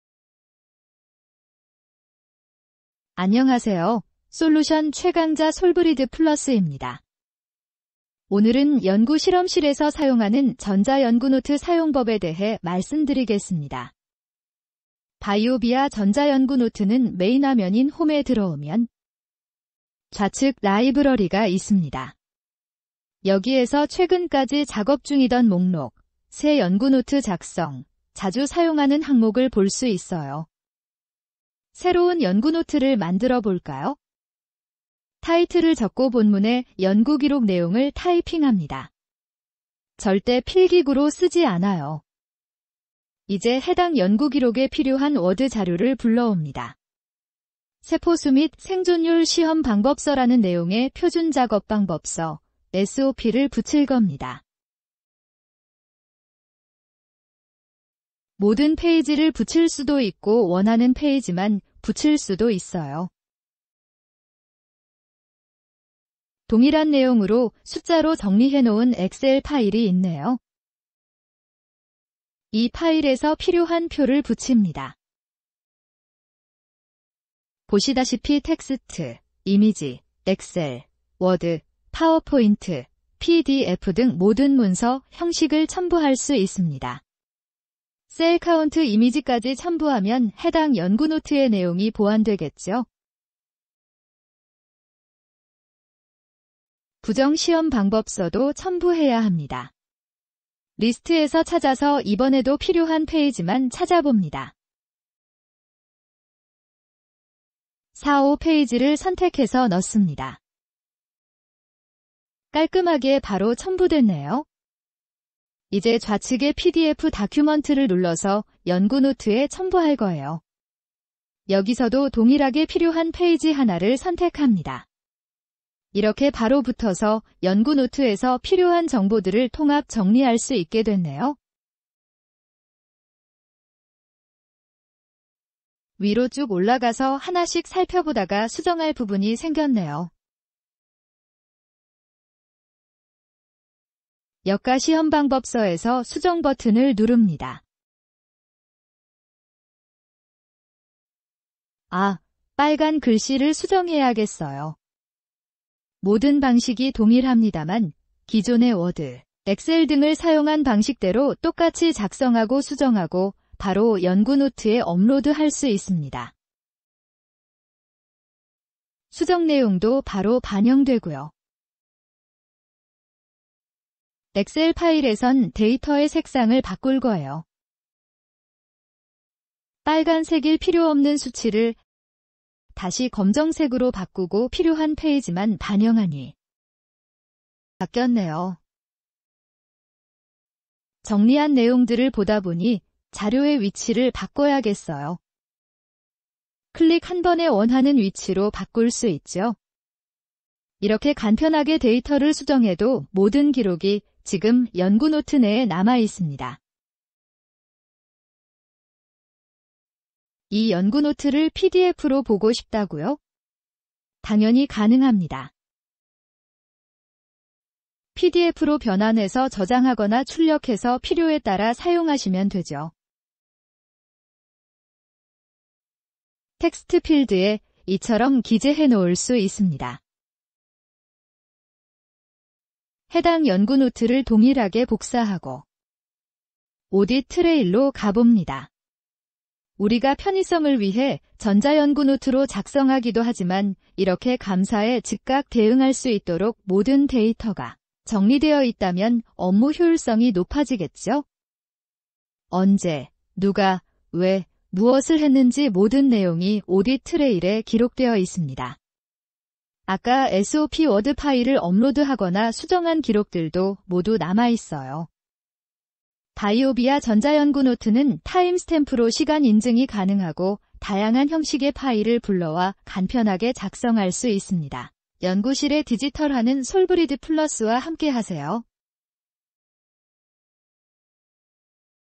[솔플 BIOVIA] 전자연구노트로 연구실 효율성 높이고 디지털화하기
바이오비아(BIOVIA) 전자연구노트를 똑똑하게 활용하여
연구실험실의 디지털화, 업무 효율성의 극대화를 누려보세요~

솔브리드플러스의 바이오의약품 전문가가 여러분과 함께합니다.🤝🏻
[솔브리드플러스 SOLBRIDPLUS]